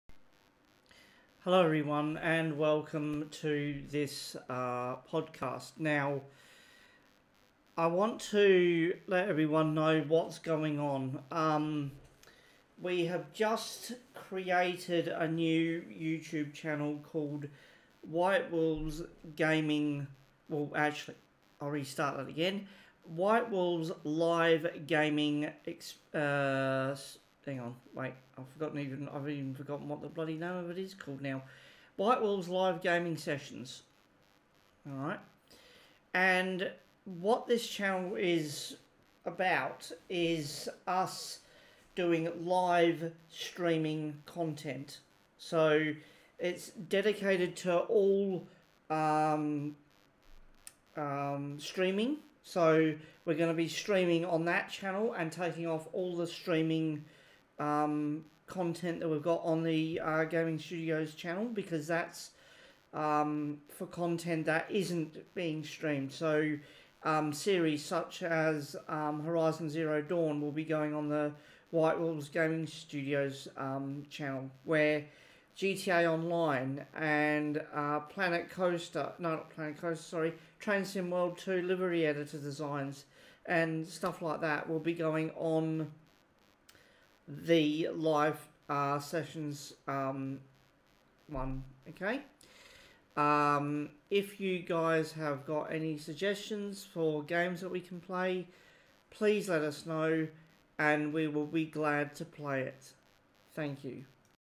Update on the channel
We are pleased to annouce we have now created our own streaming only youtube channel called Whitewolves live gaming sessions. Where we will be streaming "Select" Games.

Some of which will Be

GTA ONLINE
TSW2 - Livery editor sessions